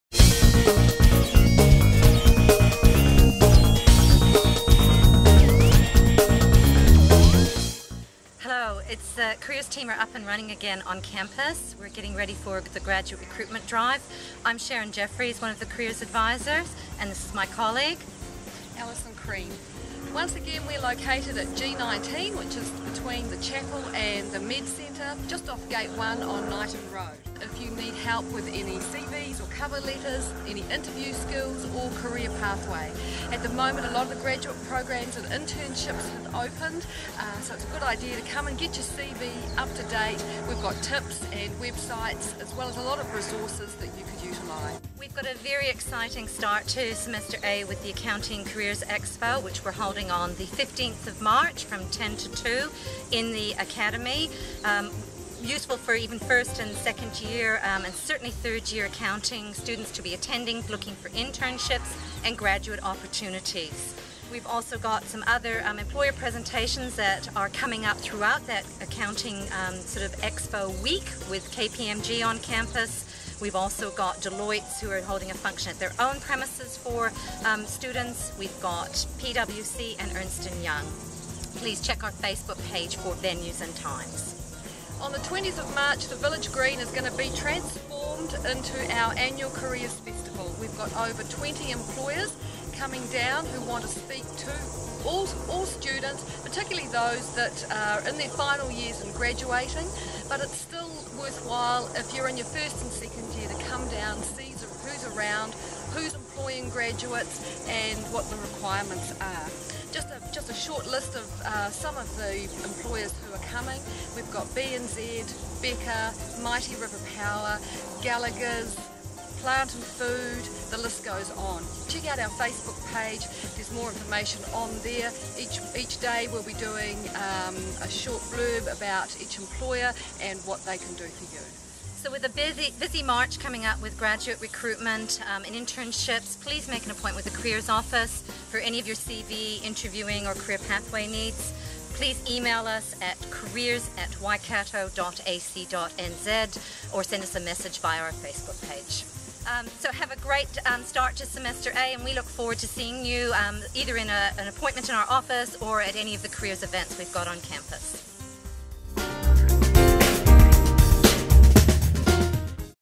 Career Services: an Introduction
We would like to welcome new students to the University of Waikato. In this video, we chat to Sharon and Alison, the friendly Careers Advisers from the University of Waikato Careers & Employment Centre, about the free careers service available for students on campus. They also talk about the upcoming careers fairs and employment presentations about to take place on campus. Don't miss these events!